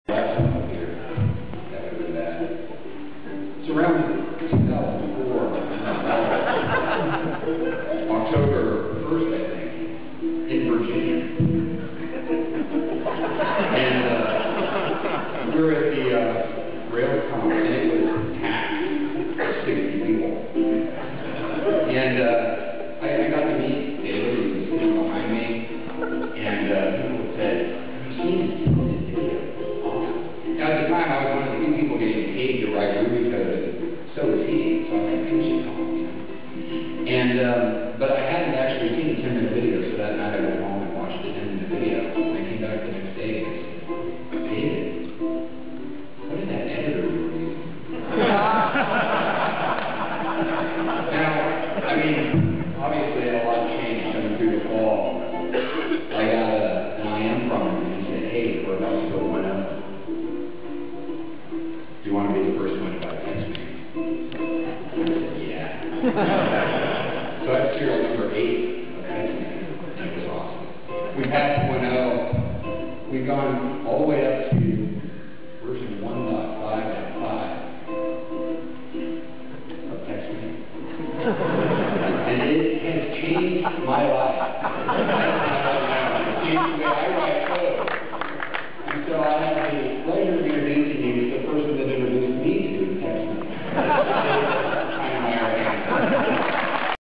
Rich Kilmer introducing DHH at RailsConf 2007
Rich Kilmer performs a very funny introduction to DHH's keynote. Banjo sound track by Chad Fowler.

Moved over from Google Video. Originally uploaded 5/18/2007